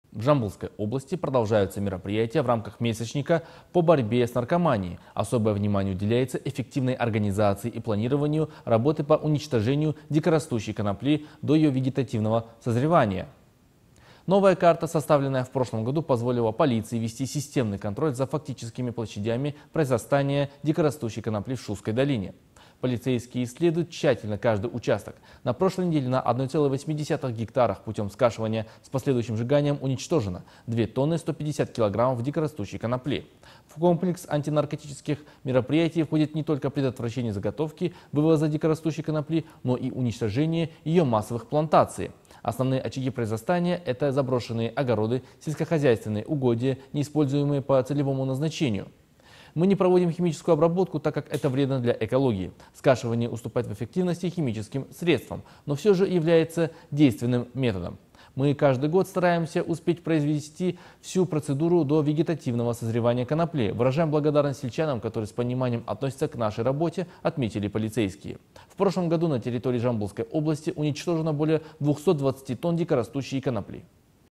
Уничтожение дикорастущей конопли
В Жамбылской области продолжаются мероприятия в рамках месячника по борьбе с наркоманией. Особое внимание уделяется эффективной организации и планированию работы по уничтожению дикорастущей конопли до ее вегетативного созревания.